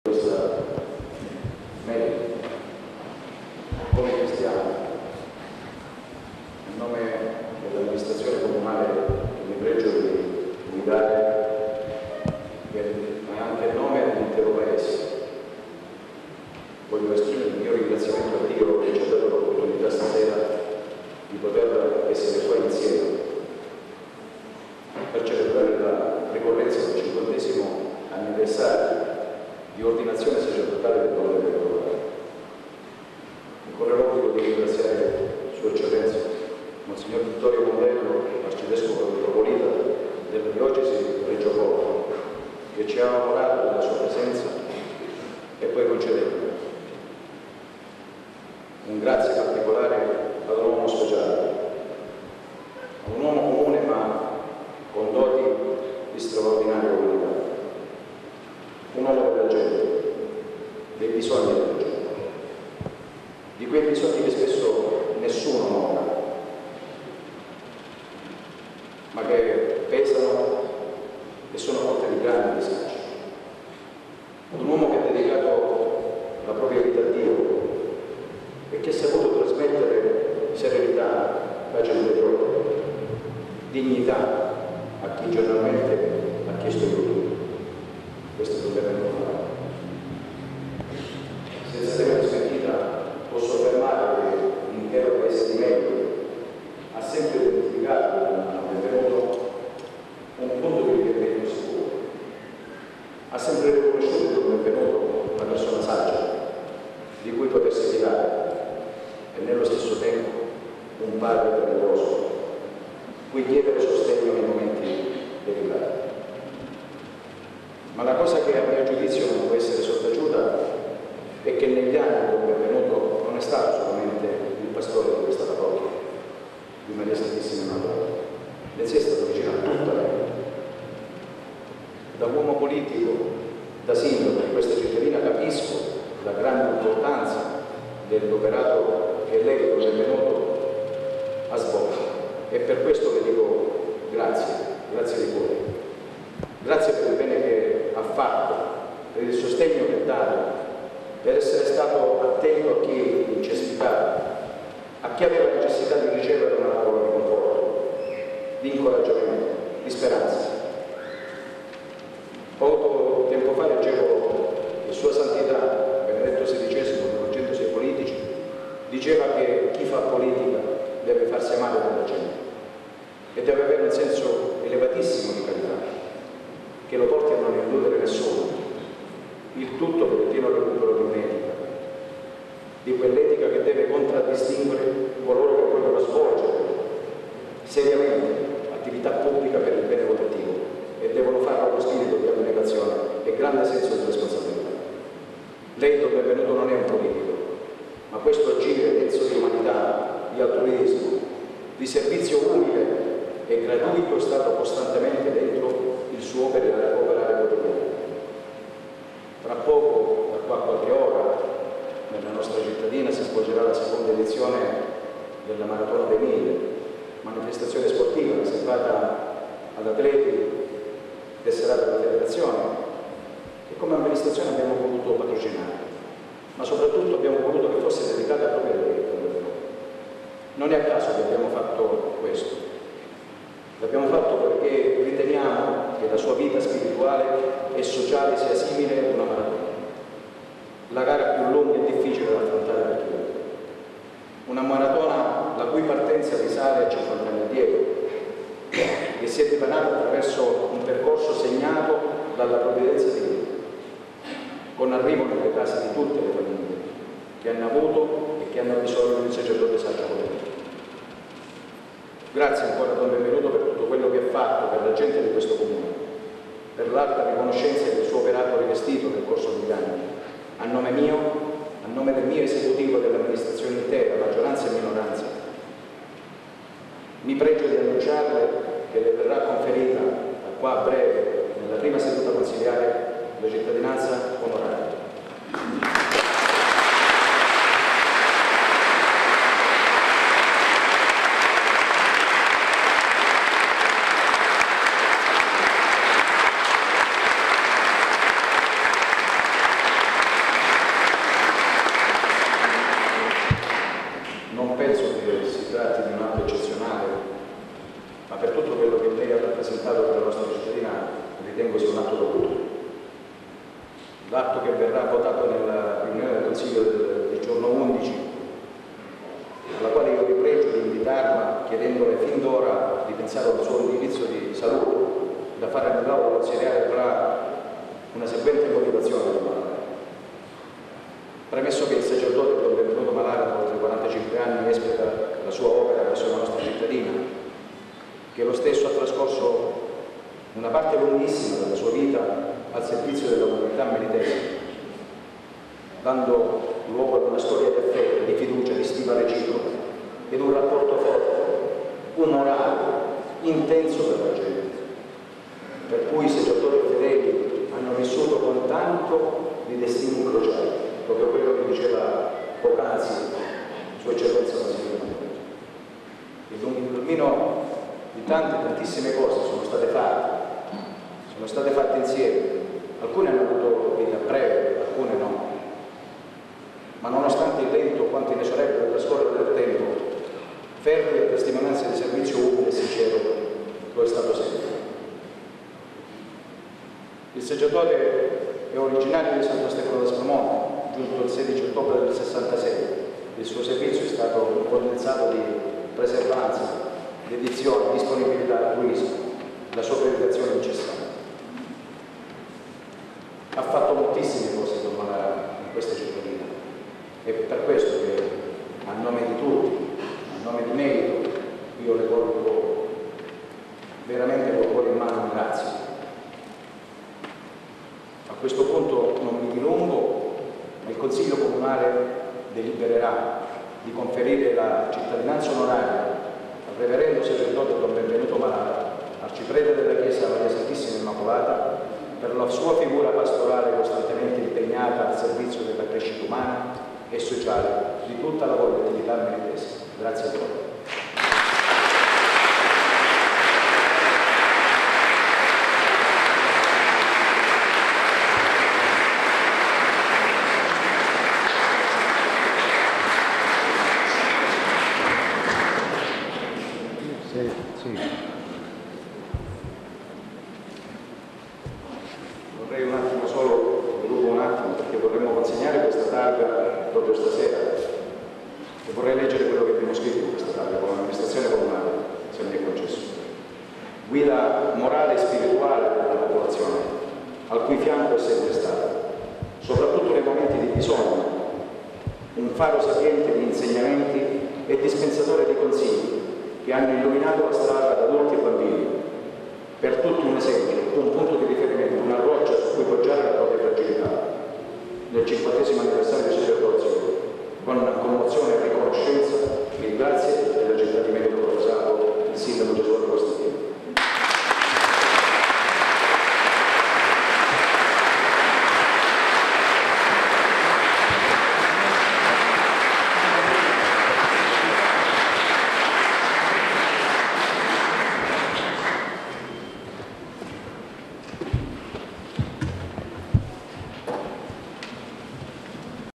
Gesualdo Costantino al 50esimo di Ordinazione Sacerdotale di Don Benvenuto Malara
Melito di Porto Salvo (Reggio Calabria) 8 luglio 2012 - Il Sindaco Gesualdo Costantino al 50esimo di Ordinazione Sacerdotale di Don Benvenuto Malara